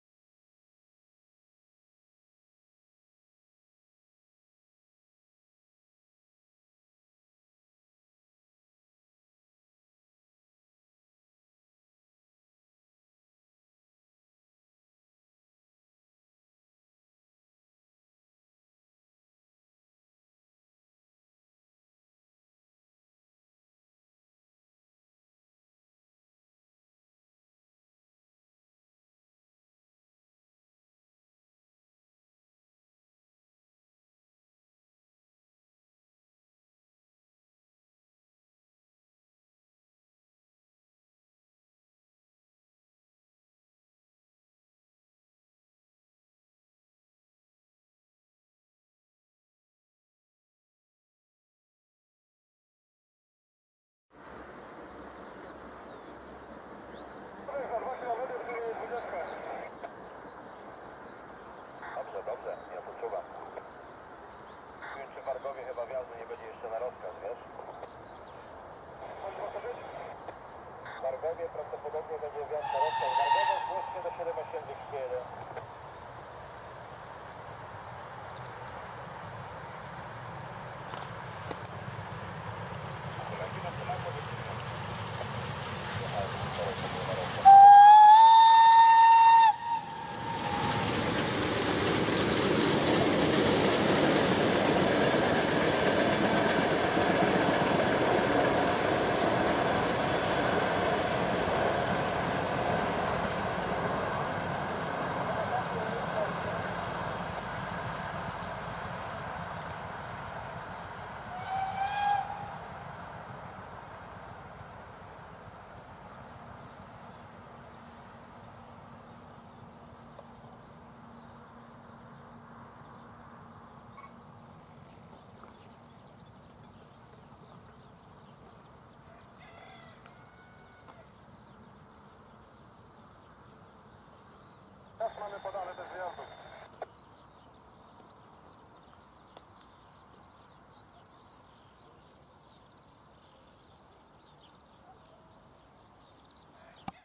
Trupy EM10 na Franowie i parowóz Pt47, 1-2.06.2019
"Wycieczka" do lokomotywowni w Poznaniu, której celem było obejrzenie lokomotyw wymarłej serii EM10, a także paru innych trupów. Następnie parowóz Pt47 pędzi wczesnym rankiem niedaleko miejscowości Oborniki z pociągiem retro "Pirat" do Kołobrzeg. Z tyłu doczepiona lokomotywa BR 285.